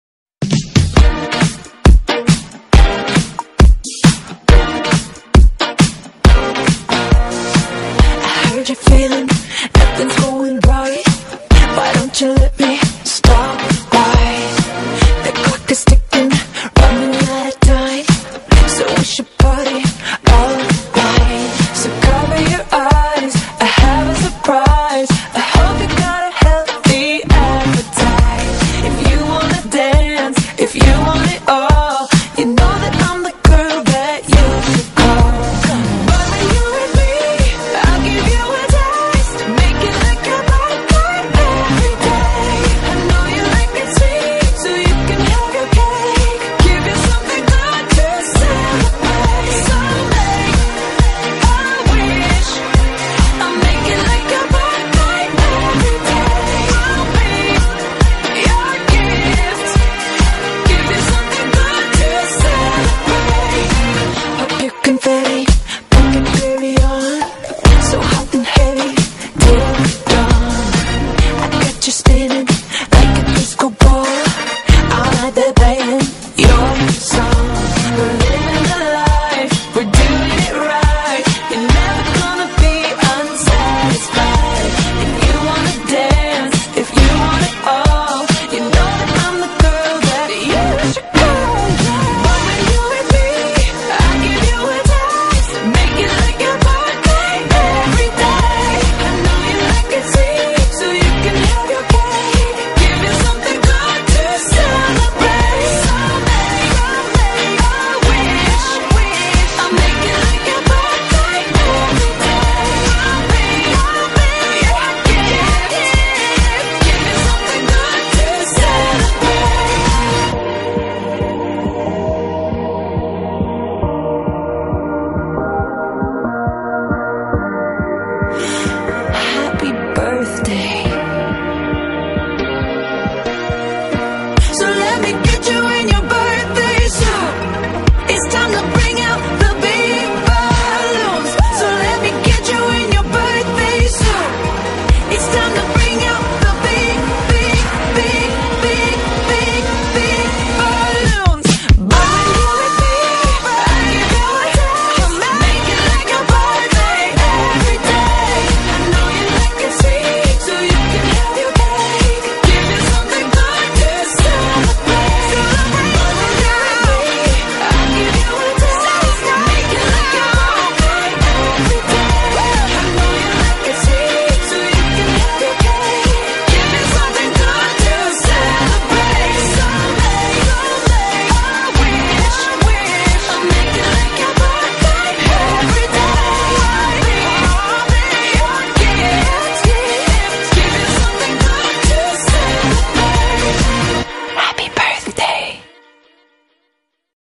Happy Birthday Katy Perry
This is my first stop motion video :) and my very first vid on youtube, hope u like it :D